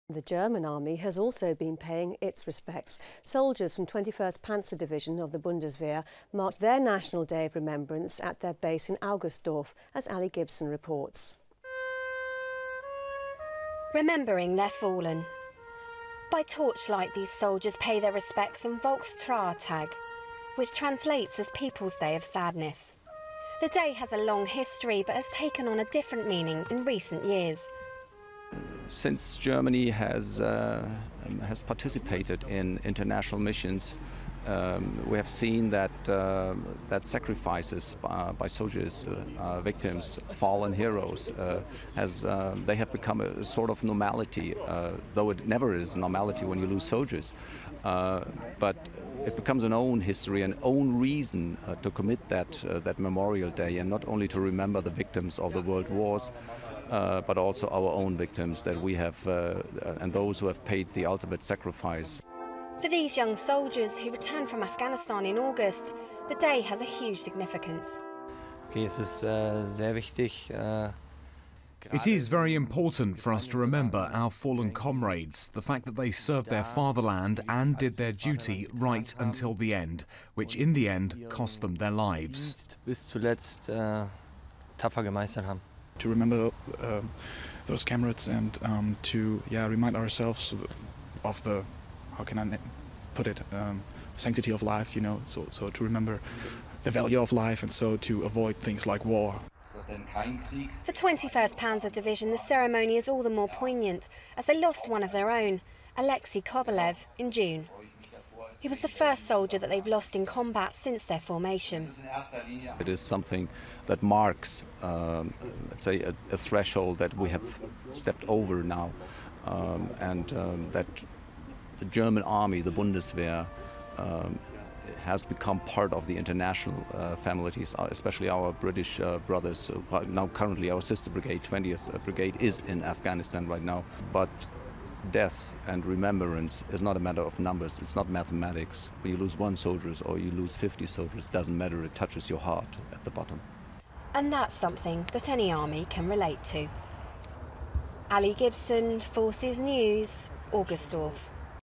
Germans mark their day of remembrance 14.11.11
Whilst Remembrance Sunday was being marked across the forces world, in Germany soldiers from 21st Panzer Division of the Bundeswehr were also marking their national day of remembrance.

The servicemen and women paid their respects on Volkstrauertag, which translates as people's day of sadness.

For 21st Panzer Division the ceremony was all the more poignant as they lost one of their own, Alexij Kobelew, in June this year.

He was the first soldier that they have lost in combat since their formation.